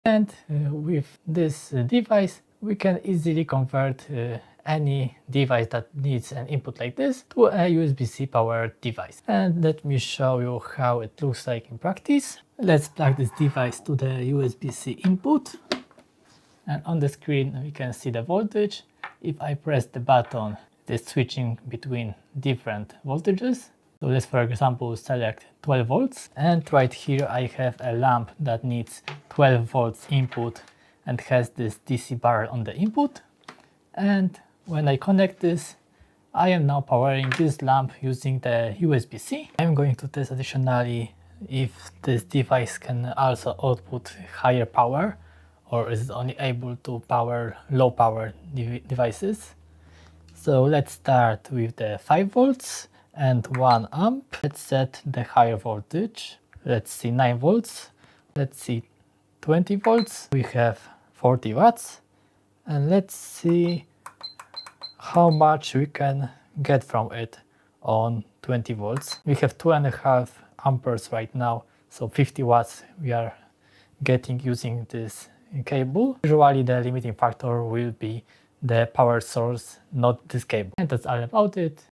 USB-C to DC Barrel Converter Demonstration
A USB-C to DC barrel converter converts various devices to USB-C power. The video tests its power output capabilities and shows its functionality.